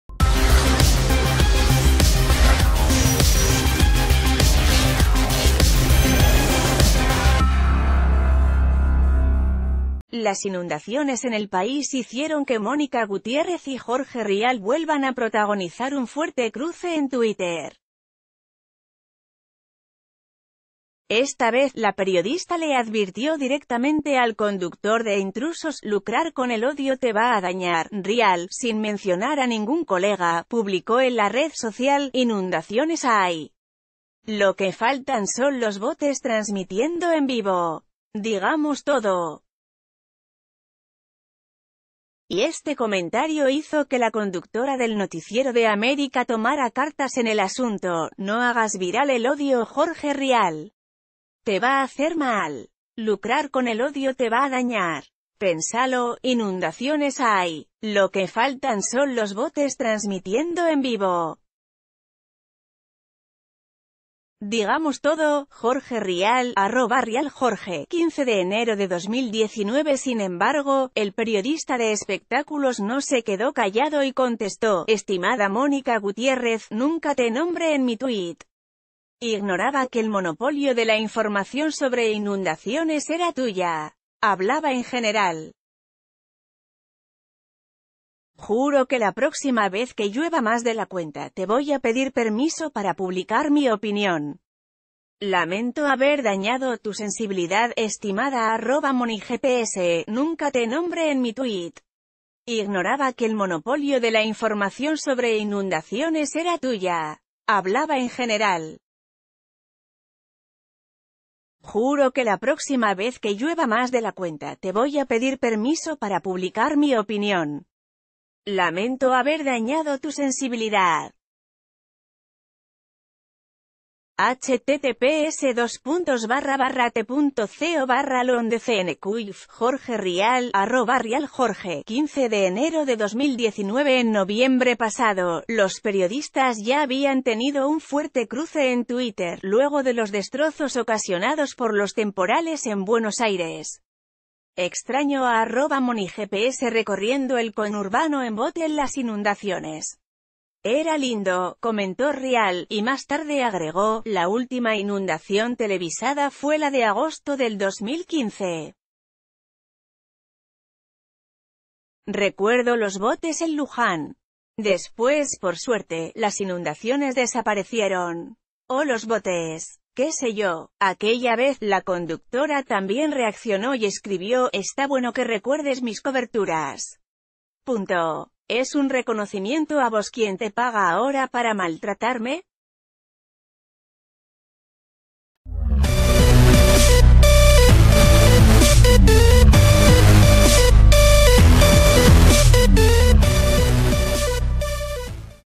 Nuevo cruce entre Mónica Gutiérrez y Jorge Rial: "Lucrar con el odio te va a dañar", lo increpó ella
Las inundaciones en el país hicieron que Mónica Gutiérrez y Jorge Rial vuelvan a protagonizar un fuerte cruce en Twitter. Esta vez, la periodista le advirtió directamente al conductor de Intrusos: "Lucrar con el odio te va a dañar".Rial, sin mencionar a ningún colega, publicó en la red social: "Inun...